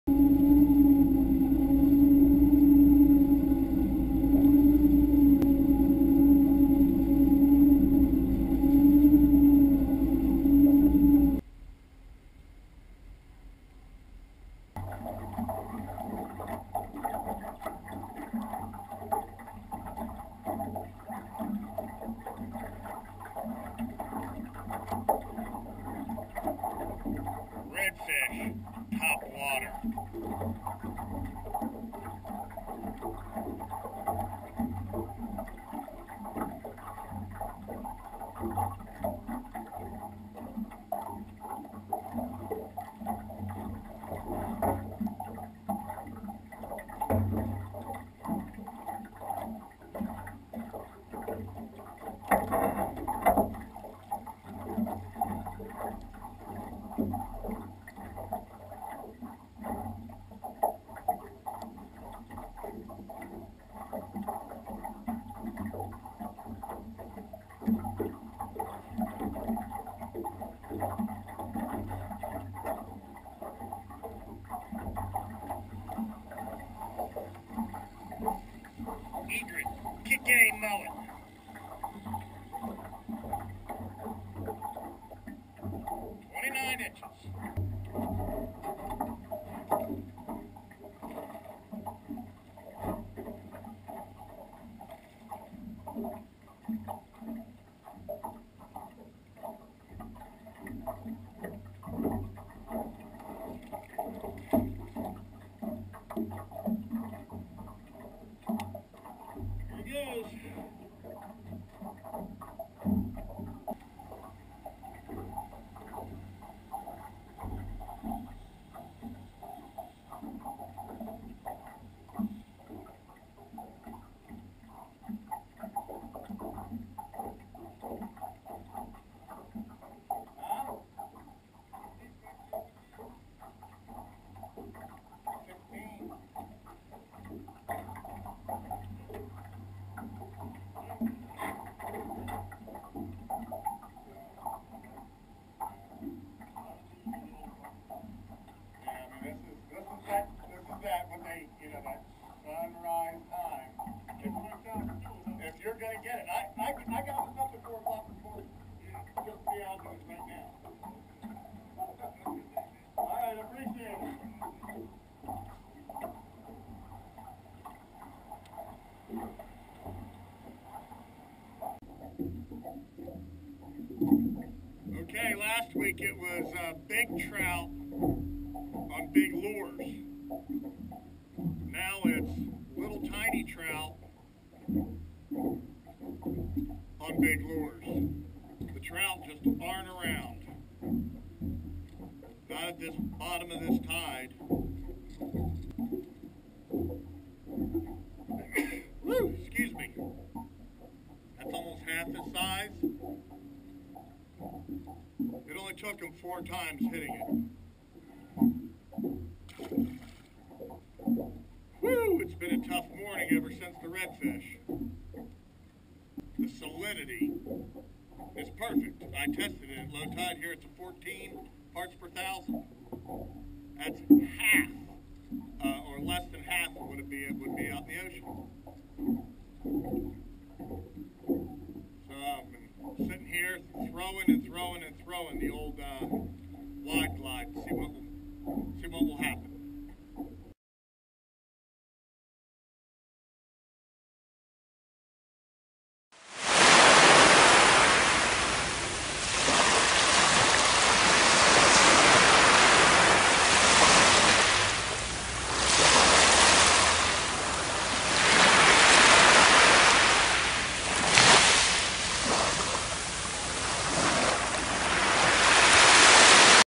How to ask FWC to check back later.....
Capt Dave Sipler Sport Fishing, since 1996 your full-time Jacksonville area fishing guide.

"JETTYWOLF LIVE"- done in 1080-HD

 - Redfish on top water. Then, not many Trout.
"Hmmm, something changed from last week!"